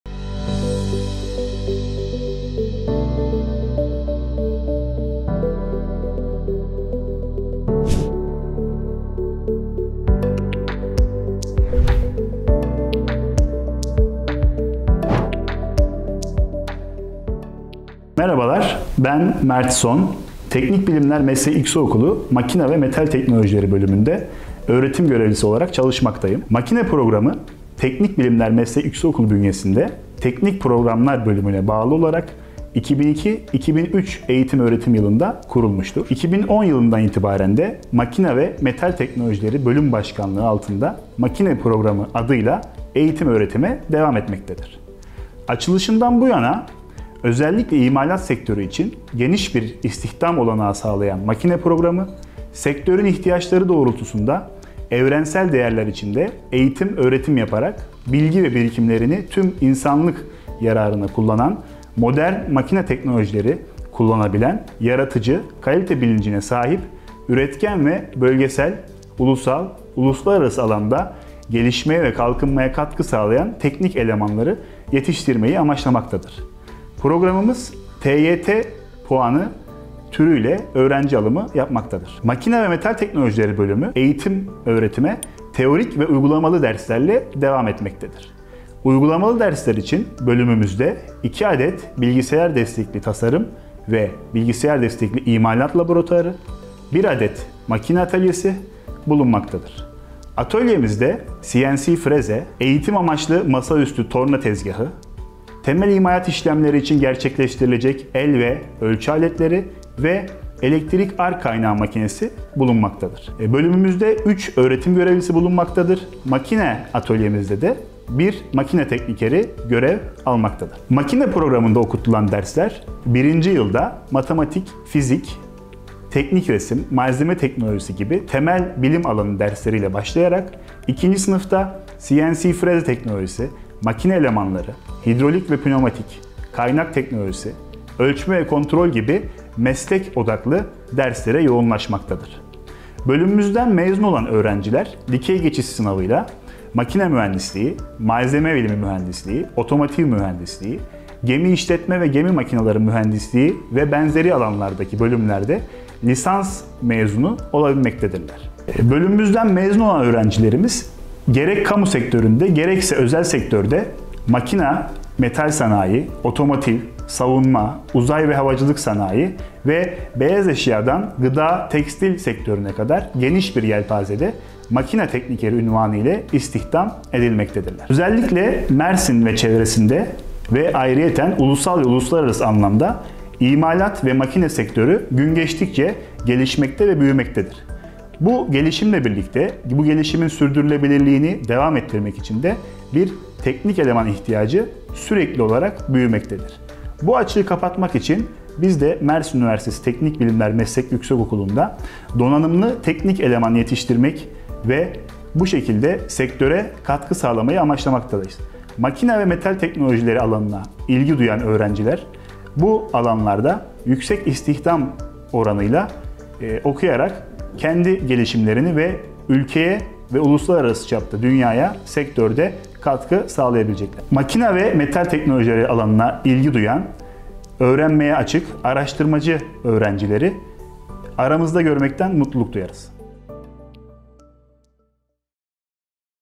Mersin Üniversitesi | Teknik Bilimler Meslek Yüksekokulu – Makine Programı
Mersin Üniversitesi'nin fakülte, yüksekokullarındaki bölümleri ve meslek yüksekokullarındaki programları tanıyacağımız serimizin bu videosunda, Teknik Bilimler Meslek Yüksekokulu Makine Programı ile ilgili bilgileri Öğr. Gör. Mert Son aday öğrencilerimize aktardı.